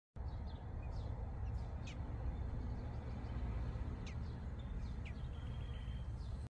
American Robin Babies 🐣 Sweet Slumber 💤 Nestling Trying to Stay Awake 🧡
American Robin Babies Sleeping | Sweet Slumber in Nature's Cradle
American Robin Newborn Nestling
American Robin Nest Close-Up
American Robin Nest in Blackberry Bush